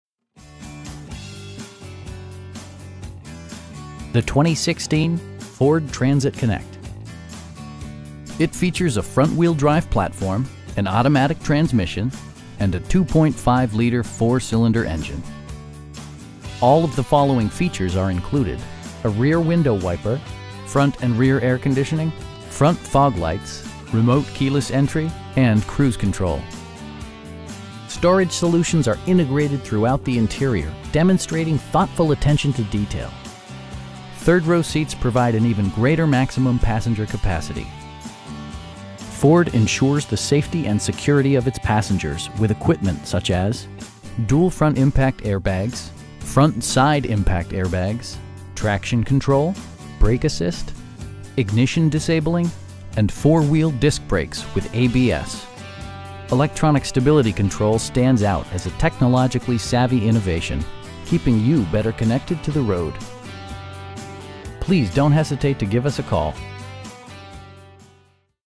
2016 Ford Transit Connect in Moline, IL 61265
Sexton Ford
3802 16th Street

The 2016 Ford Transit Connect. It features a front-wheel-drive platform, an automatic transmission, and a 2.5 liter 4 cylinder engine. All of the following features are included: a rear window wiper, front bucket seats, front and rear air conditioning, front fog lights, power windows, remote keyless entry, and cruise control. Storage solutions are integrated throughout the interior, demonstrating thoughtful attention to detail. Third row seats provide an even greater maximum passenger capacity. Ford ensures the safety and security of its passengers with equipment such as: dual front impact airbags, front side impact airbags, traction control, brake assist, ignition disabling, and 4 wheel disc brakes with ABS. Electronic stability control stands out as a technologically savvy innovation, keeping you better connected to the road. We have the vehicle you've been searching for at a price you can afford. Please don't hesitate to give us a call.